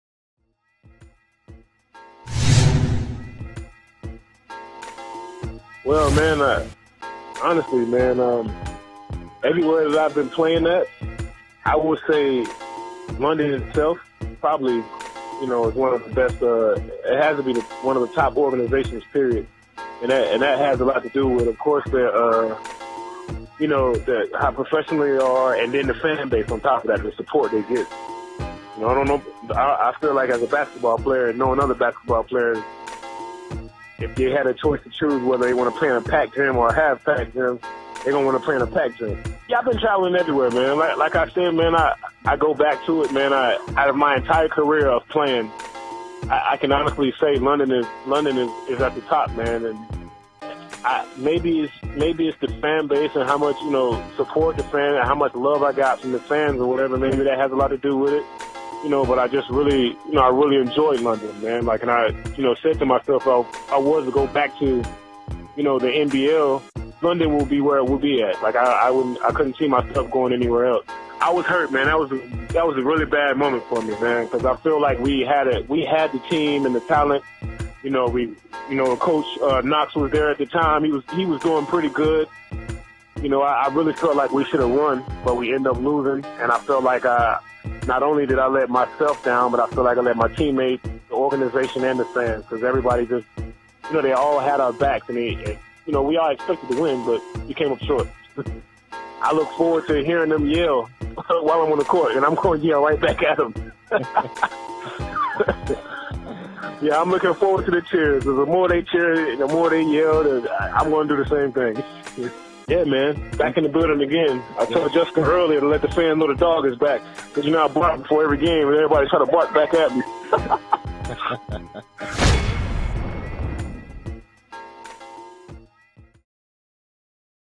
Marvin Phillips Returns to London Lightning! | NBL Canada
The London Lightning is thrilled to welcome Marvin Phillips back to London.

“Marvin is going to be an impactful addition to our team. We feel we have put together a strong group and his experience, energy and talent level is only going to make us stronger,” said head coach Kyle Julius.

Phillips is a 6’7 forward from Jacksonville, North Carolina who played with the London Lightning for three seasons before accepting an offer to play in France. During his time with the London Lightning he averaged 15 points and 10 rebounds per game and was named 2013 playoff MVP, the same year the Lightning won their last championship in 2013.

Phillips is also the London Lightning’s all-time leading scorer and rebounder.